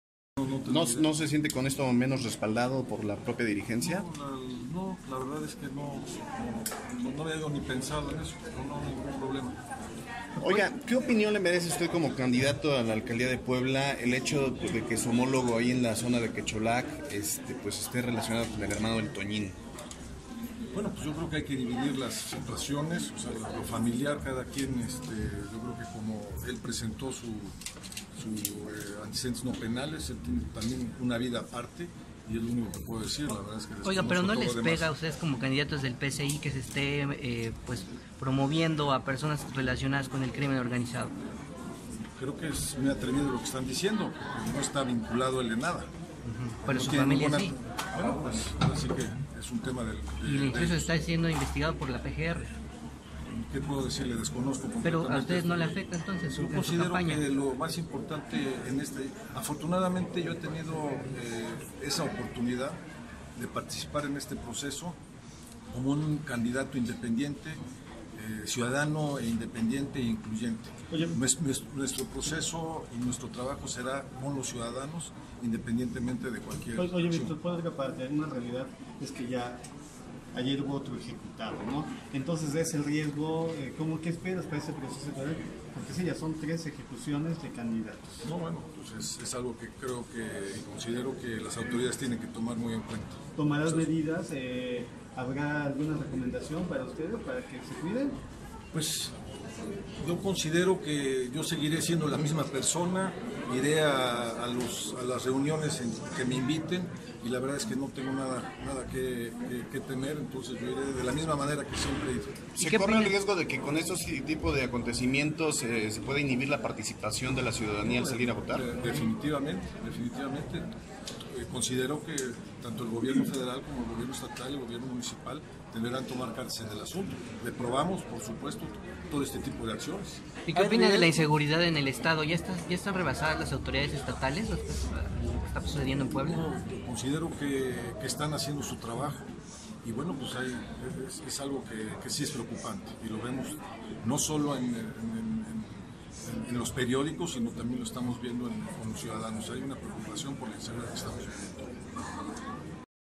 Víctor Gabriel Chedraui se registra como candidato de PSI a la alcaldía de Puebla
Se registra Víctor Gabriel Chedraui como candidato de PSI a la alcaldía de Puebla; afirma que tienen un proyecto que beneficiará a la ciudad.